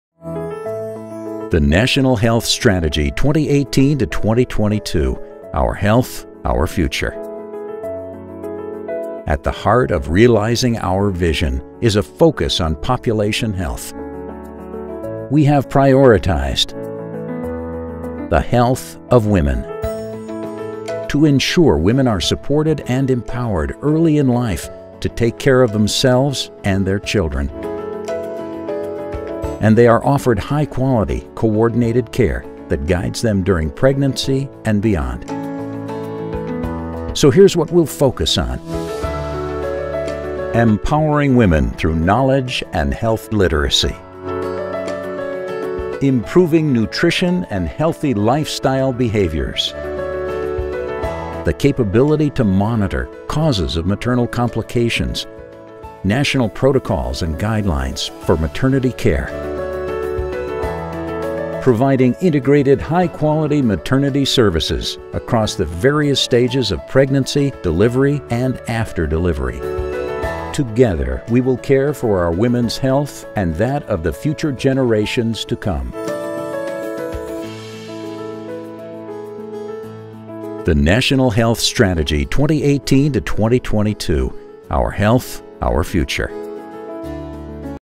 Healthy Women Leading to Healthy Pregnancies
NHS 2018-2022 Plans for ‘Healthy Women Leading to Healthy Pregnancies’ aim to improve the capability to monitor causes of maternal complications, introducing national protocols and guidelines for maternity care including infertility and caesarians, introducing a midwifery care service and providing integrated high-quality maternity services across the various stages of pregnancy, delivery and after delivery.

We will help, inform and empower women to lead healthier lives by providing them with high quality maternal care, enabling them to take care of themselves and their children.